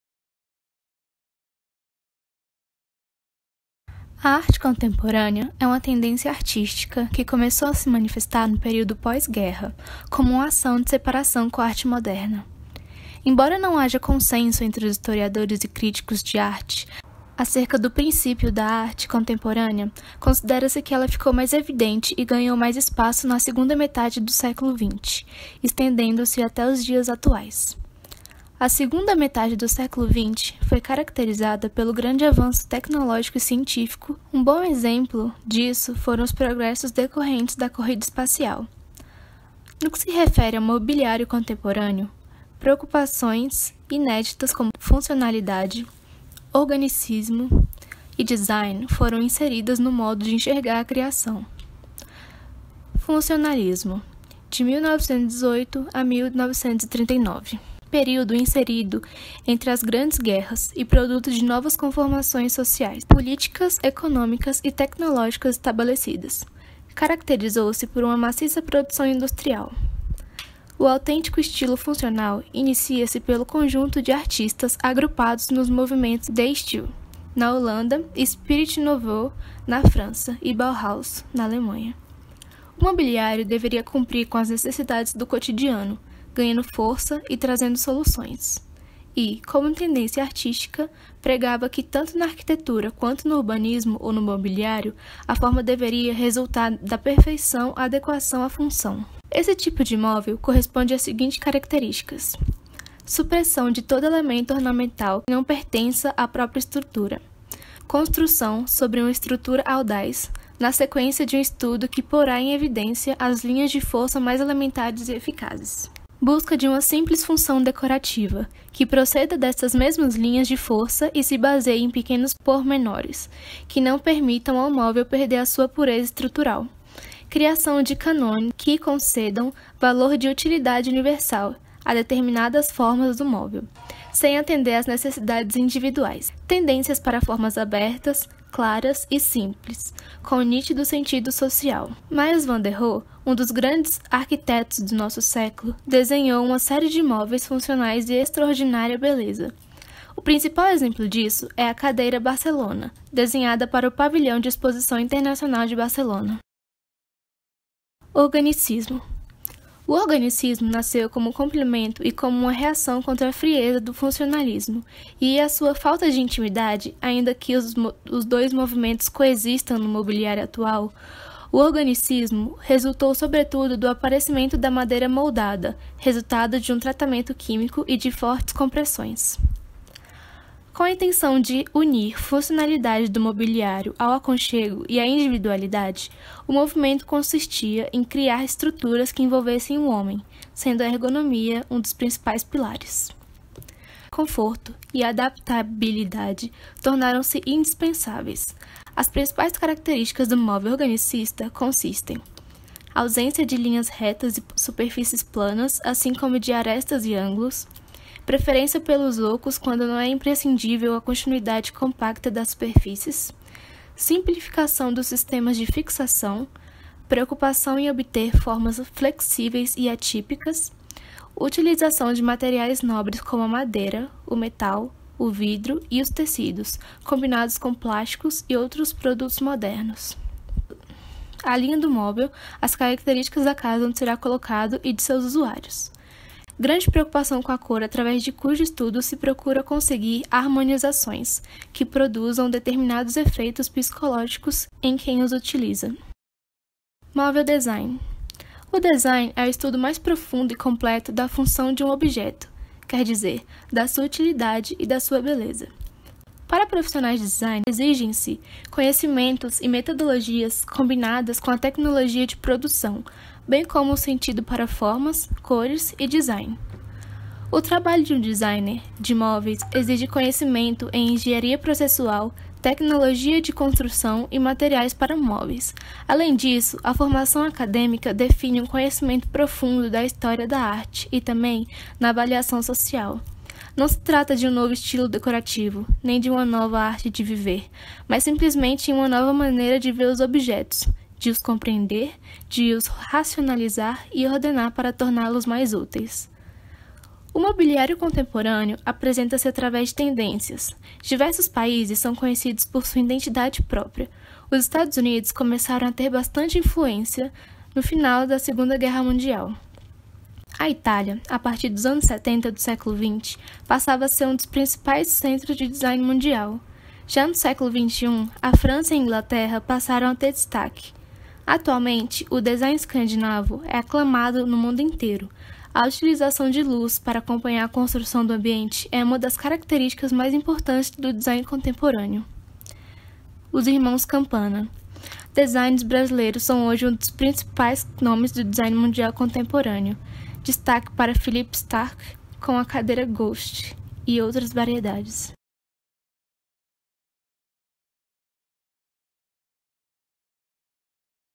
História do Mobiliário Arte Contemporânea
Vídeo produzido na disciplina de Estilo e História do Mobiliário, Ministrado pela Profa. Me. Giorgia Barreto Lima Parrião, no curso de Design de Interiores no UDF.